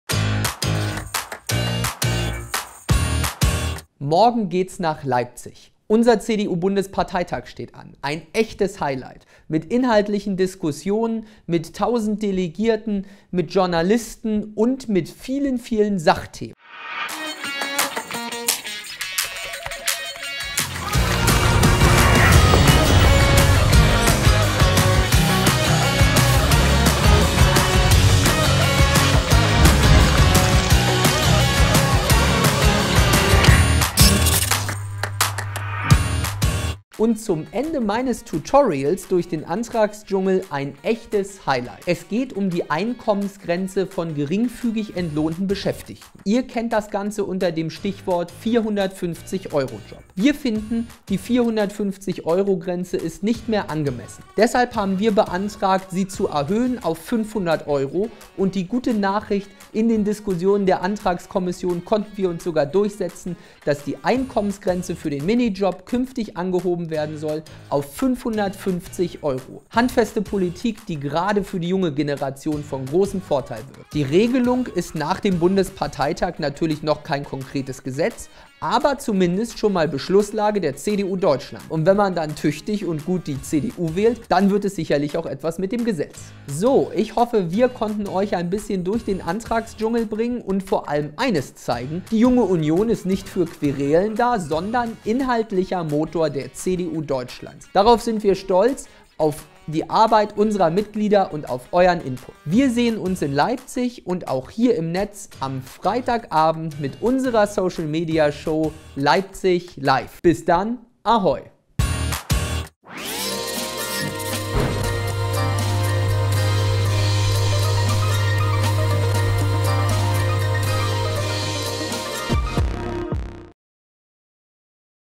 AMTHORS ANTRÄGE #4
MORGEN GEHT'S LOS!

Philipps Antrag Nummer 1 hat echte Auswirkungen auf deinen Geldbeutel. Schau rein! Morgen sehen wir uns bei Leipzig! LIVE! Einschalten! :)